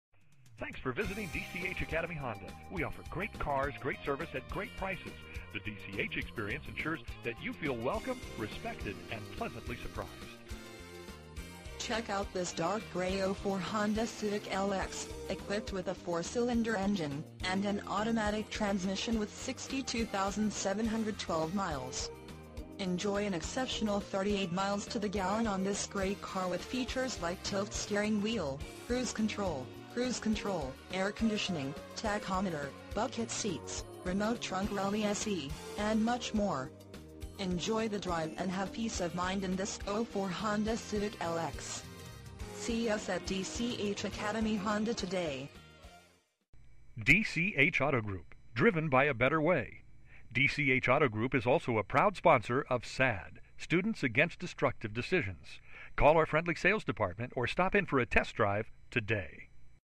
SOLD - 2004 Honda Civic LX 08857 Dch Academy Honda
1101 Route 9

2004 Honda Civic LX

 62,712 Miles

VIN - 2HGES16594H525294

Imagine driving this dark  Gray 2004 Honda Civic LX, equipped with a 4 Cyl. engine  and an automatic transmission with  62,712 miles. Enjoy an exceptional 38 miles to the gallon on this great car with features like Tilt Steering Wheel, Cruise Control, Cruise Control, Air Conditioning, Tachometer, Bucket Seats, Remote Trunk Release, and much more. Enjoy the drive and have peace of mind in this 2004 Honda Civic LX. See us a...